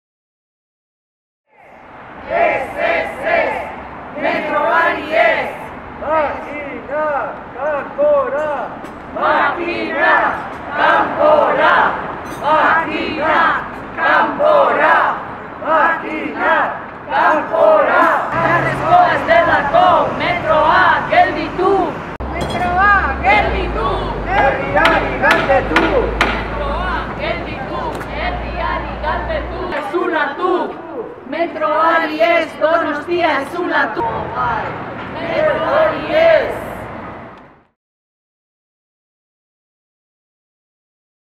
Metroaren aurkako protesta
Metroaren proiektuko azterketa lanak hasi direla salatu dute Satorralaia bizilagunen mugimenduko kideek. Erdialdeko Zaragoza plazan egin dute protesta, makinak dauden toki berean.